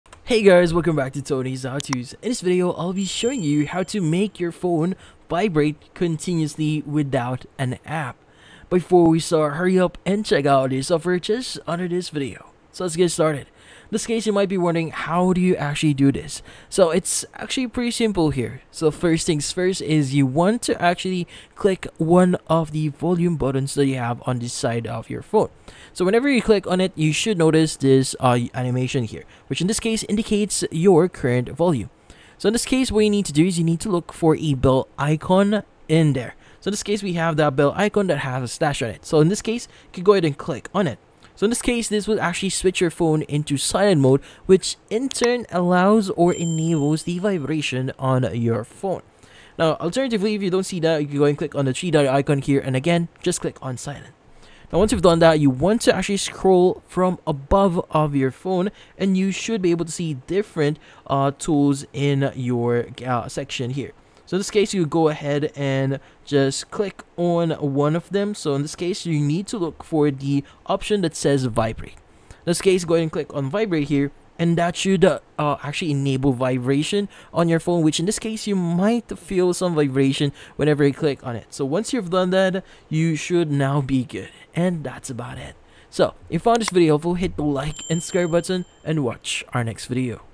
How to Make Your Phone Vibrate Continuously Without an App (2024)
In this video, I show you How to Make Your Phone Vibrate Continuously Without an App in 2024. The quick and easy tutorial how to make your phone vibrate Non Stop. Do you want to know step by step how to vibrate phone speaker for water in 2024? Yes? Great!